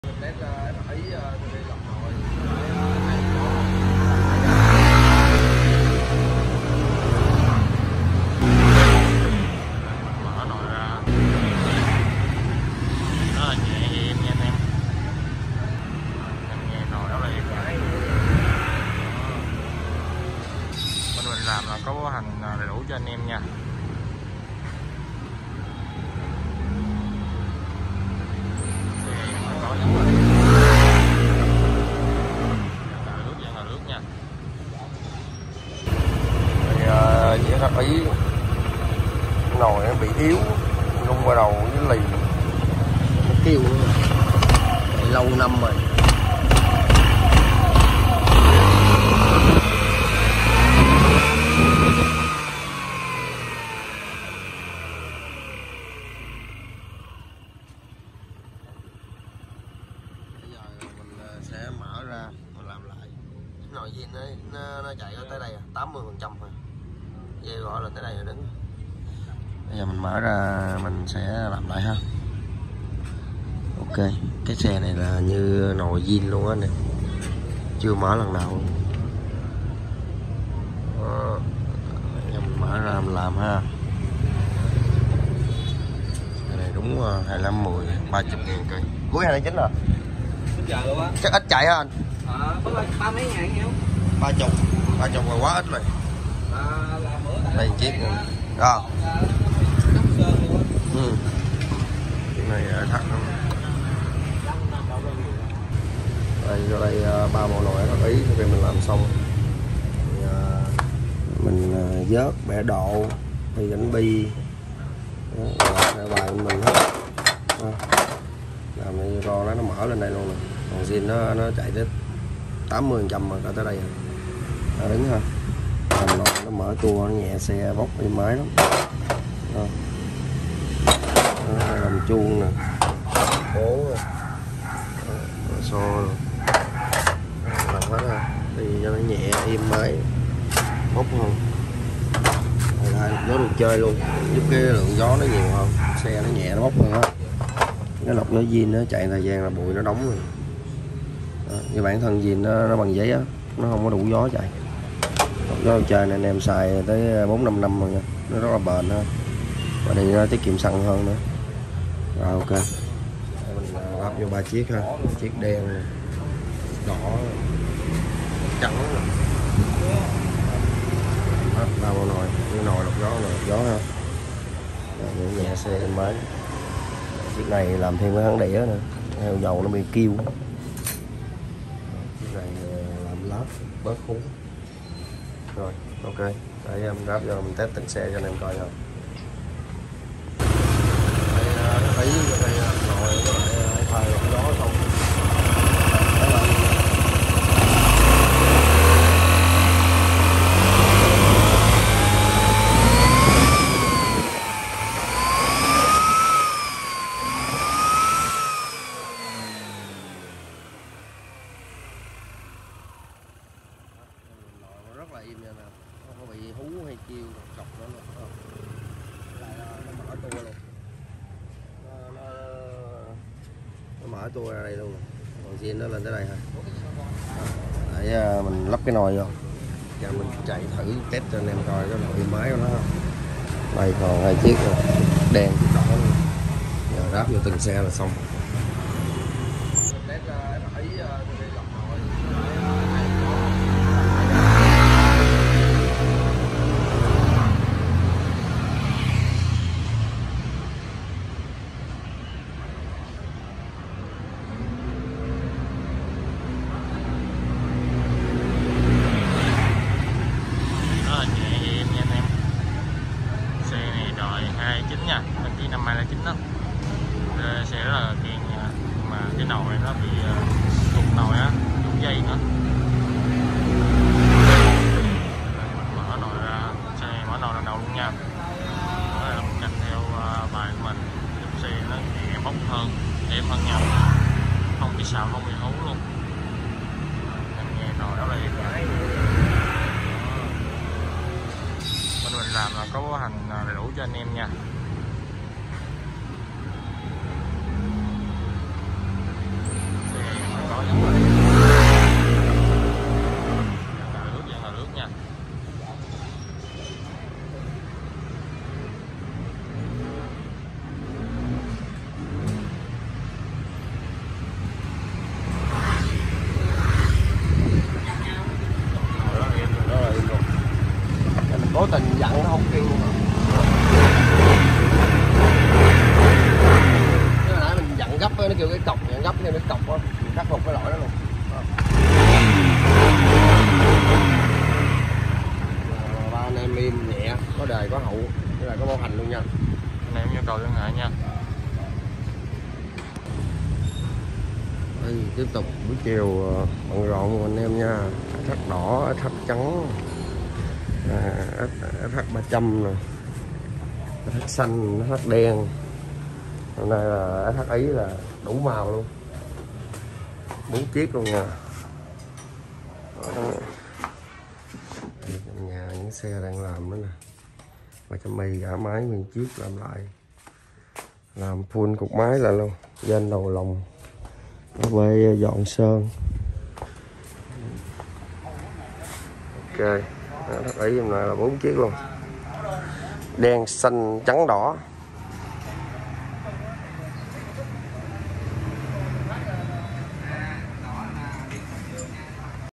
Khắc phục lỗi rung rần ga đầu lì nồi cho Sh ý giúp xe chạy mạnh bốc êm ái hơn zin rất nhìu tại TBK
Khắc phục lỗi rung rần ga đầu lì nồi cho Sh ý làm full nồi mở tua máy thay lọc gió đồ chơi giúp xe chạy mạnh bốc êm ái hơn zin rất nhìu tại TBK Racing Team 147 Tân Khai P4 Q11 Bảo Hello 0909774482 Thanks